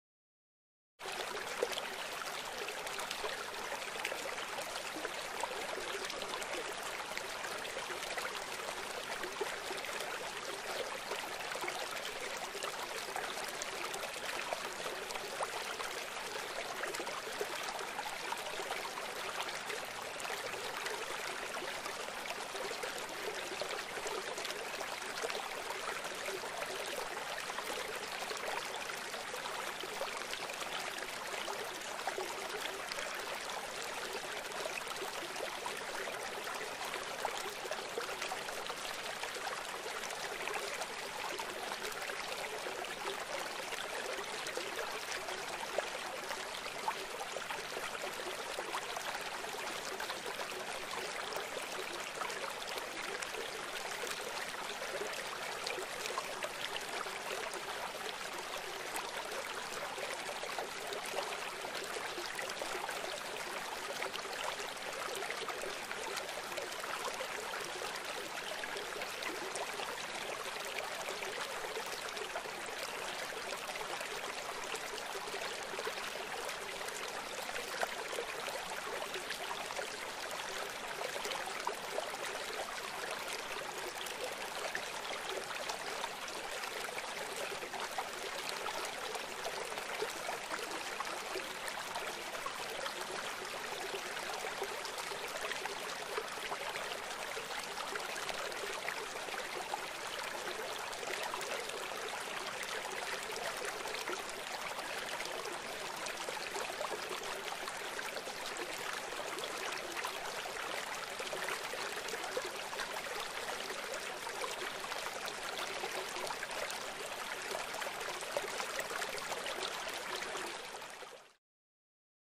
River nature sounds relaxing no birds
nature sounds relaxing water sound effect, with video of small river, filmed in Lebanon eastern mountains chain, March 25th 2020, no birds just water,
it's a short clip of river sounds..